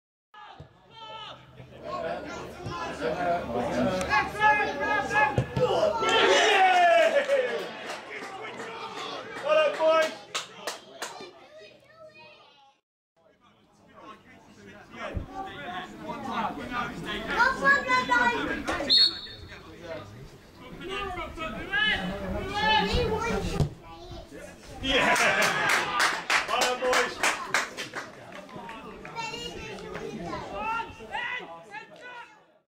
Clanfield 85 2 Shrivenham 0
Shrivvy's opening game of the 2017-18 season was a visit to Clanfield 85 (Saturday August 12, 2017) and it ended with a 2-0 defeat.